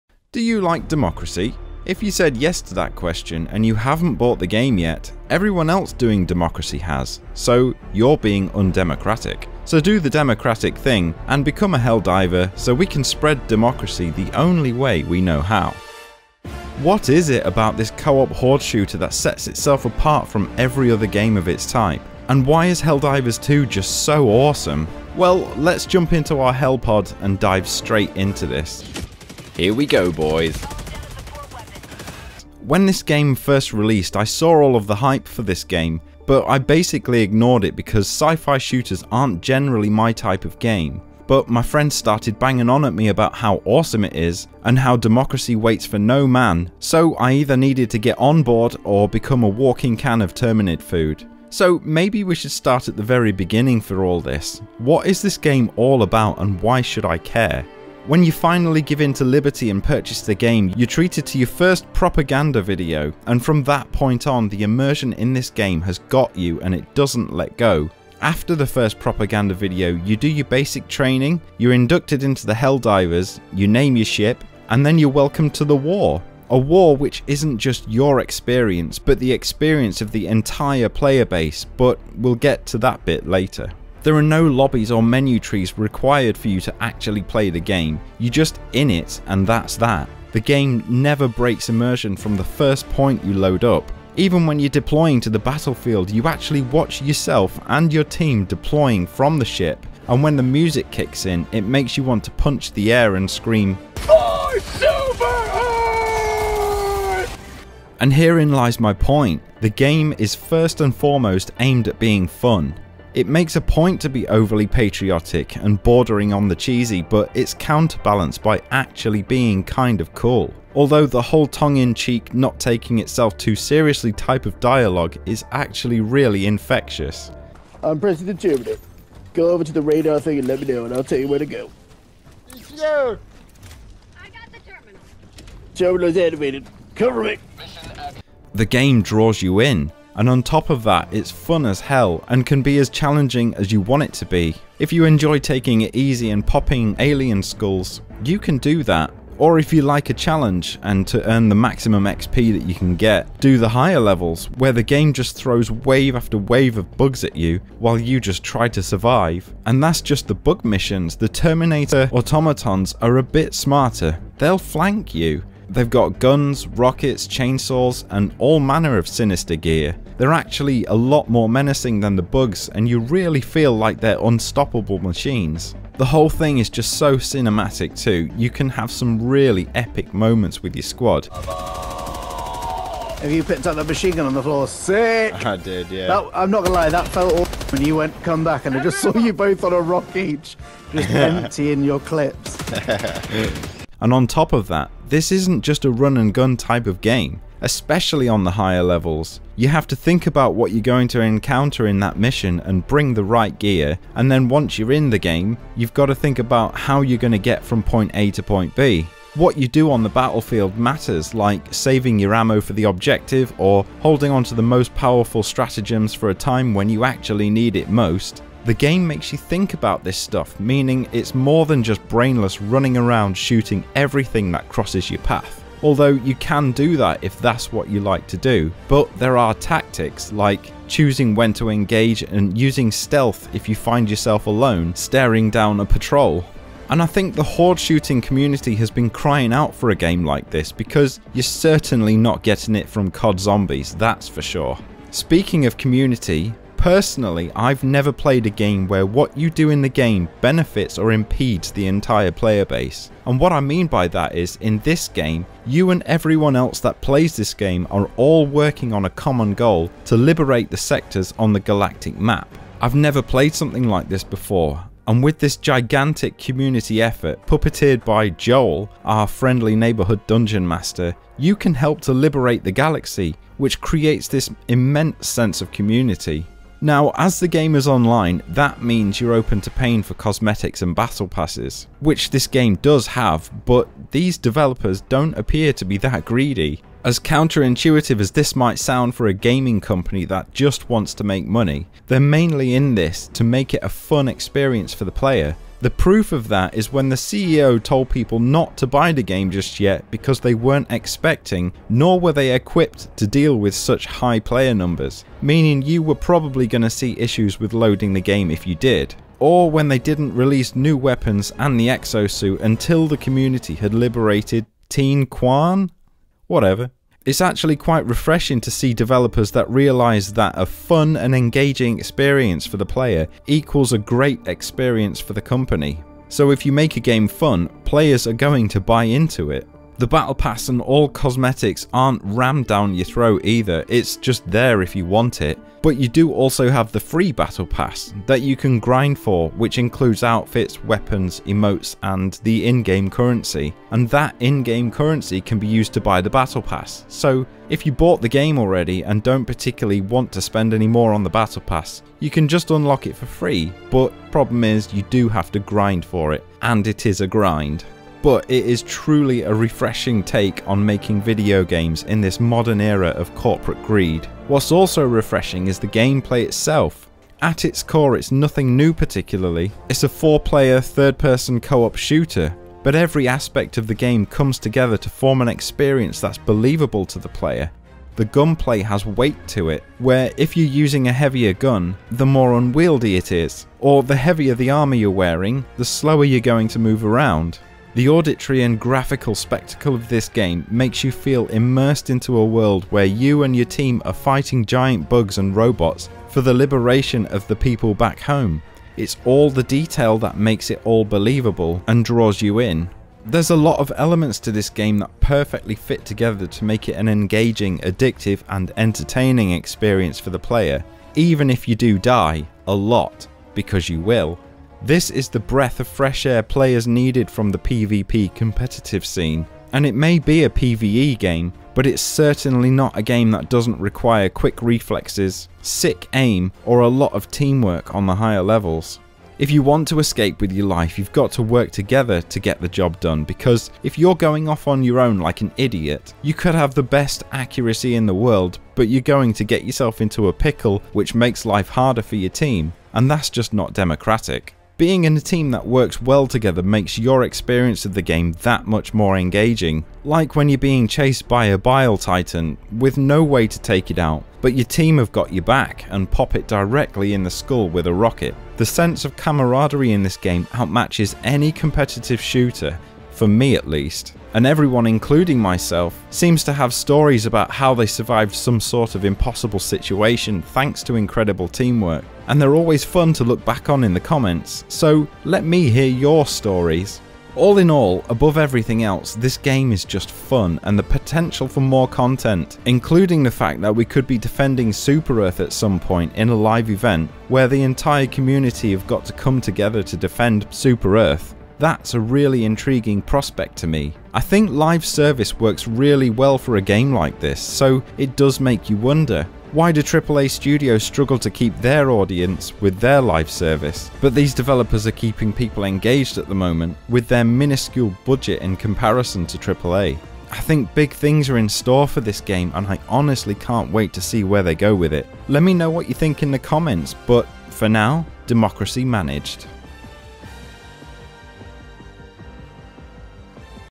Why Helldivers 2 is SO GOOD
Helldivers 2 was an unexpected masterpiece, even the developers weren't prepared for how well received this game would be. So what is Helldivers 2 and why is it SO GOOD?
In this video, I explain my thoughts on what I think makes this game so amazing, why it's blowing up and why it's just an absolute wake up call to other Live Service developers out there. Will Helldivers 2 change gaming forever, even PvE games?

- There will be playlists for all sorts of stuff so watch out!